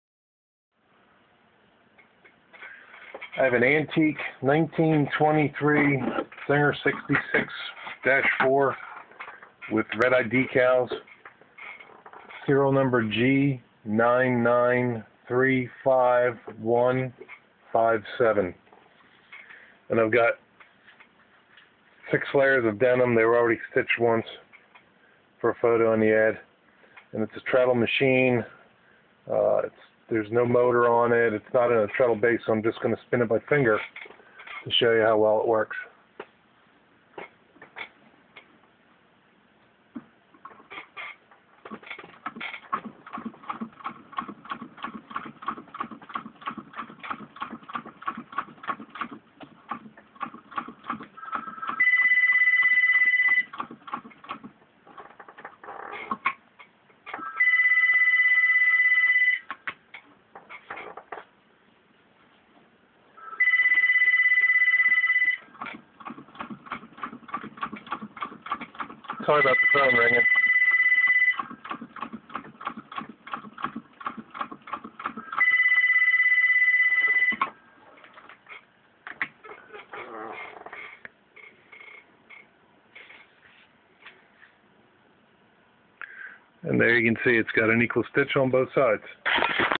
Rare Antique 1923 Singer 66-4 Red Eye Treadle Sewing Machine G9935157
This video demonstrates a Antique 1923 Singer 66-4 Red Eye Treadle Sewing Machine serial number G9935157.